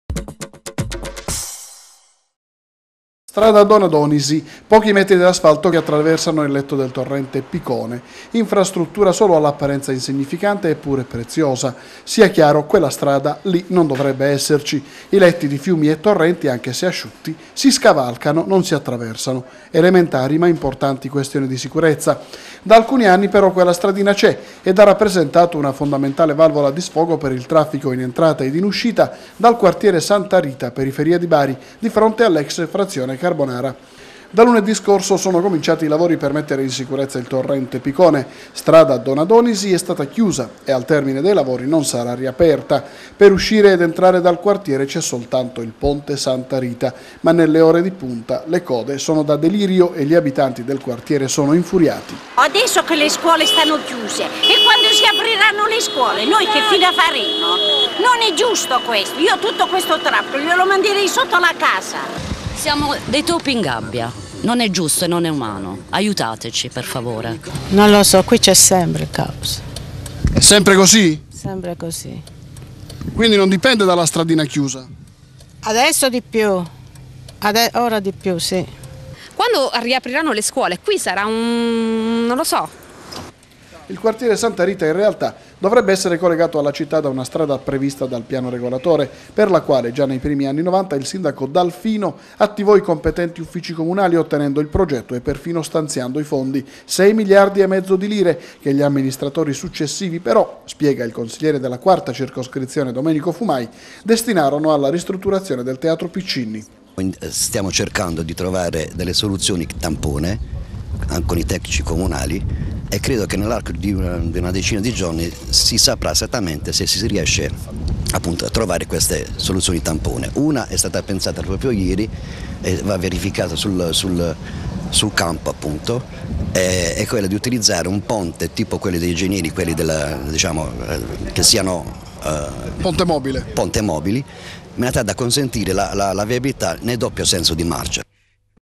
TG 17.06.10 Santa Rita, comunità in trappola?
Si sentono in trappola i 5000 abitanti del quartiere Santa Rita a Bari dopo la chiusura, per i lavori di messa in sicurezza del torrente Picone, di una strada che consentiva il collegamento con il resto della città. Ora è attiva  una sola via di fuga e le code sono infinite.